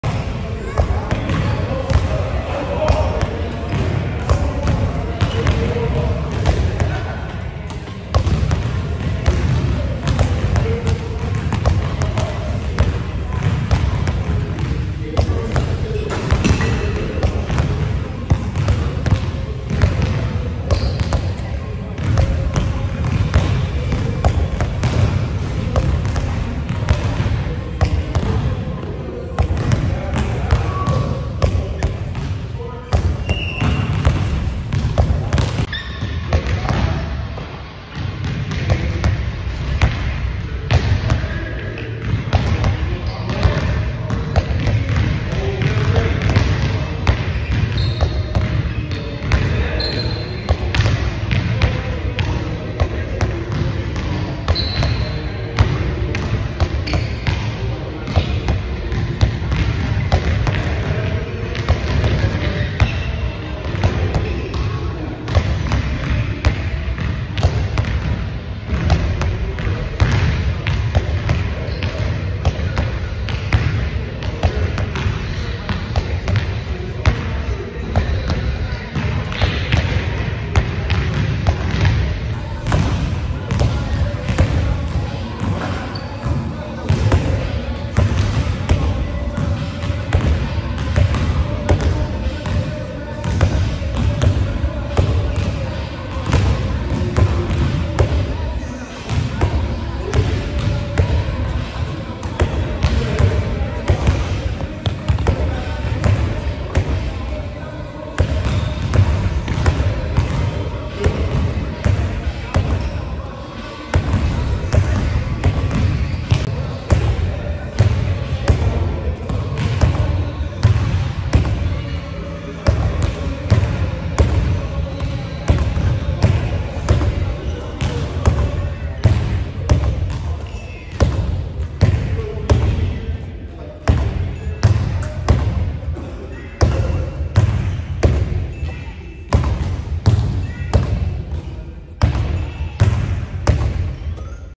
How To Become A Better Passer
Find a wall or a partner and get to work. Passing the basketball is a very important part of the game.